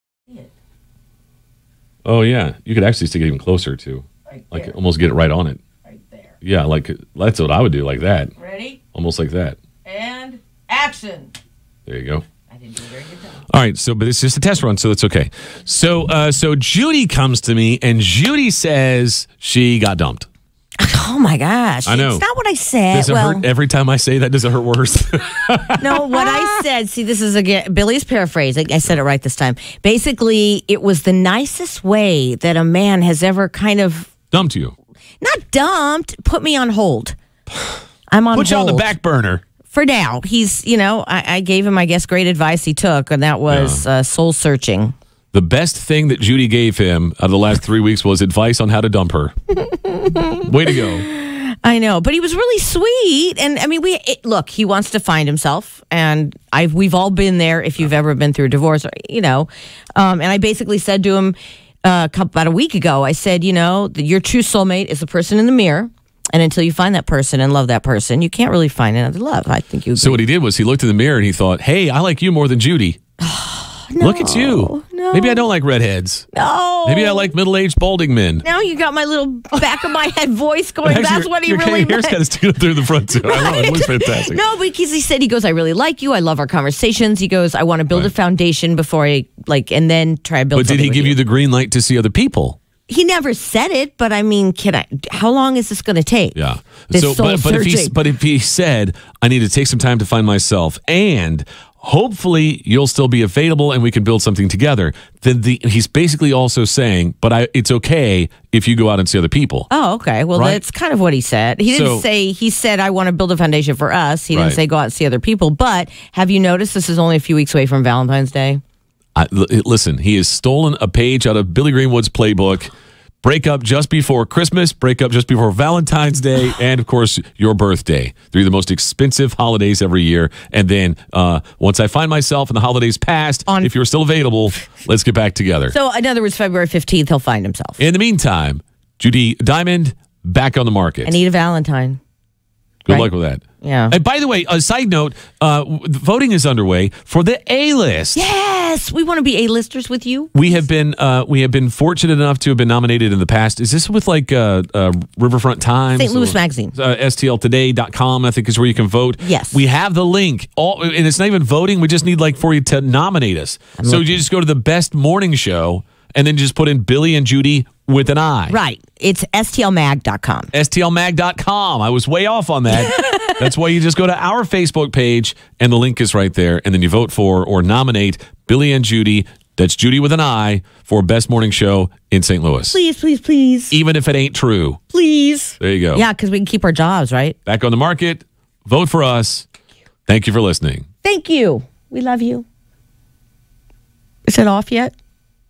Billy translates the Truth To Judi about Her Guy
Billy is translating Judi's Love Life. Is he right?